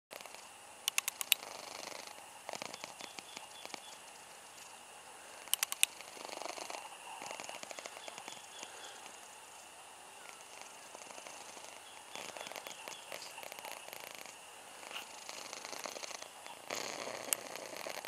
Smokey the purring machine
My beautiful boy Smokey was a purring machine.